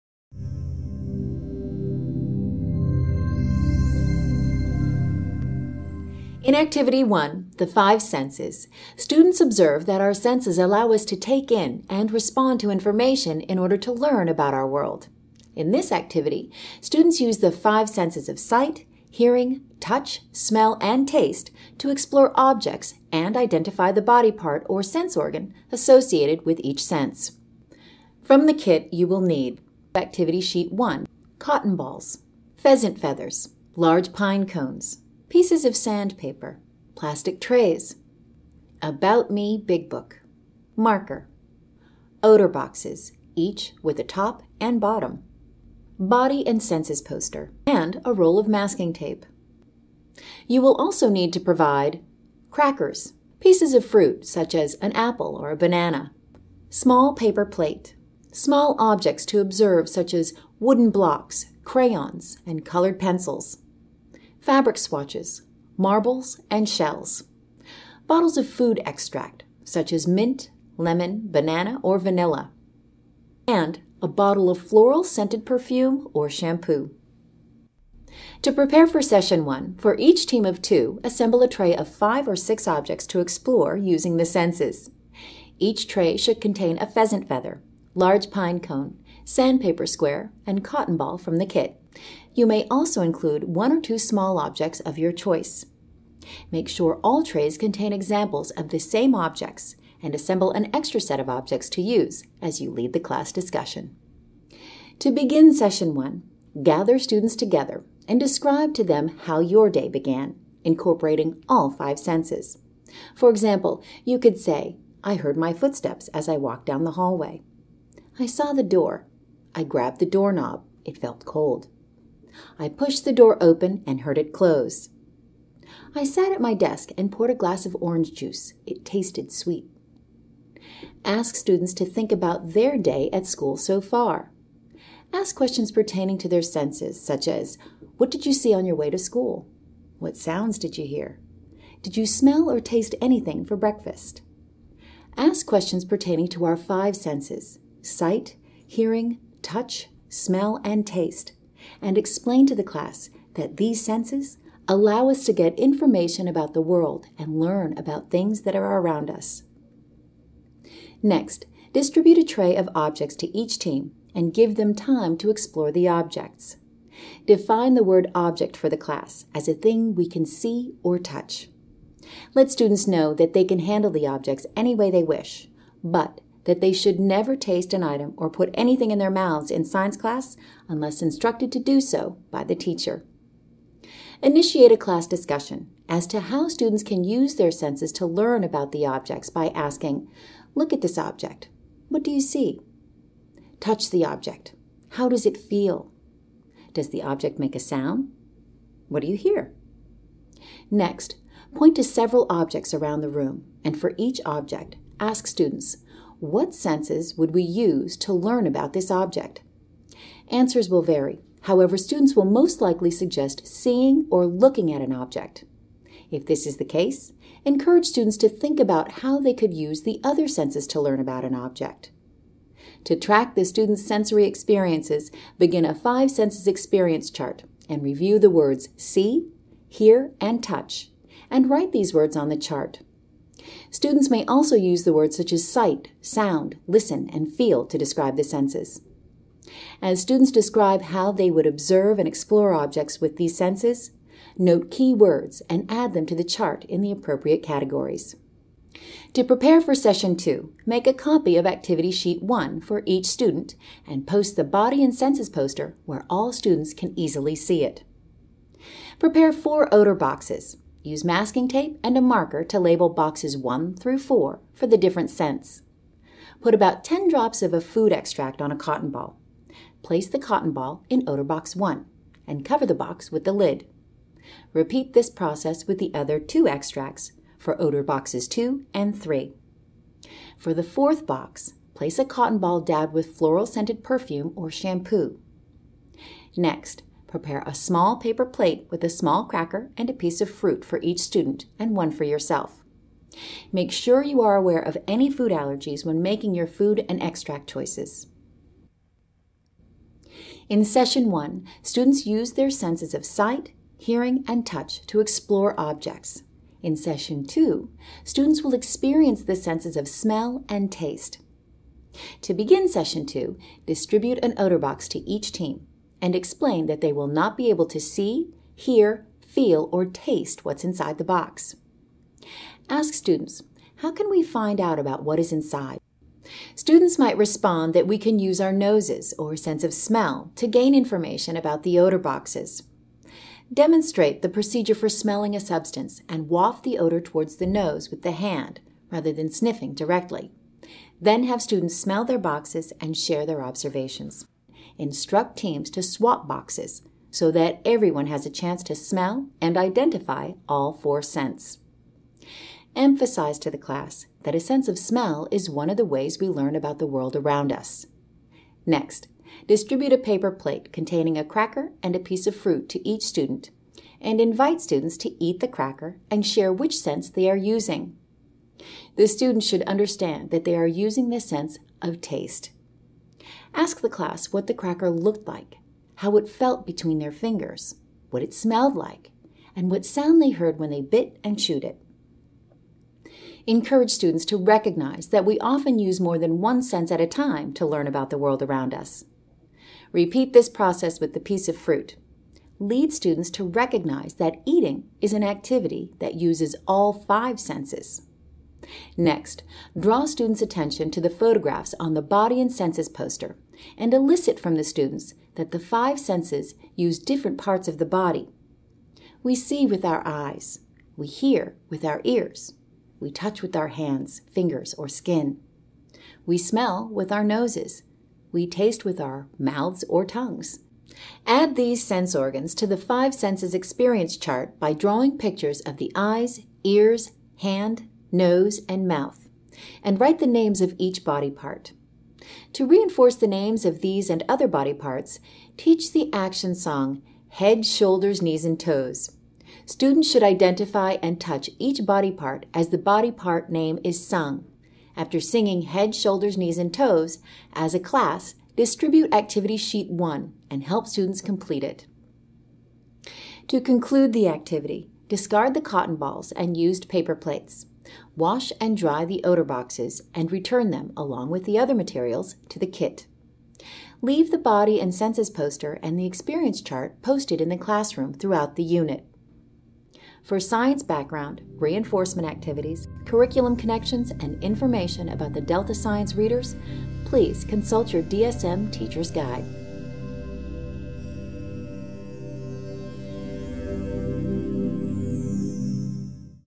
How Do We Learn? - Activity 1: The Five Senses
Students observe that the five senses allow us to take in and respond to information in order to learn about our world. They use the senses of sight, hearing, touch, smell, and taste to explore objects, and they identify the body part—or sense organ— associated with each sense.